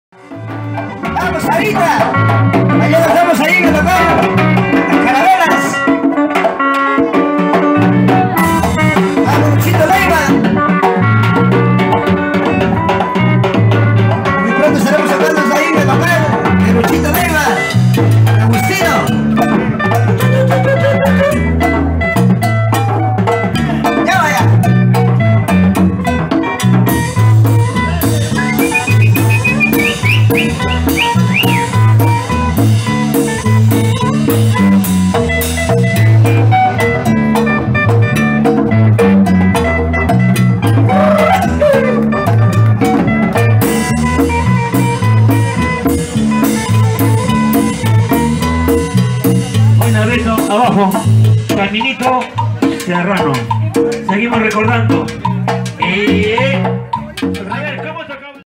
El baile del chevere Felix Martinez R.
Felix Martinez el chevere en el cumpleaños de Hugo Navarro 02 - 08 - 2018